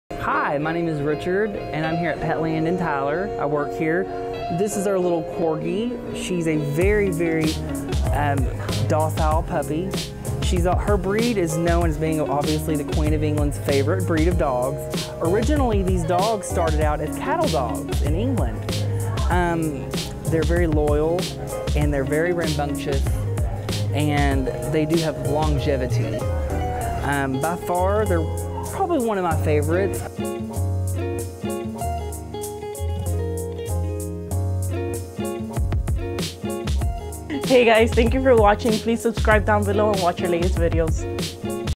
Here's how the Pembroke Welsh Corgi stole our Pet counselor's heart! | Petland Texas
Bold, friendly and playful, the Pembroke Welsh Corgi is as sweet as a peach! Here's how this breed stole our Pet counselor's heart! 🐶😚🐾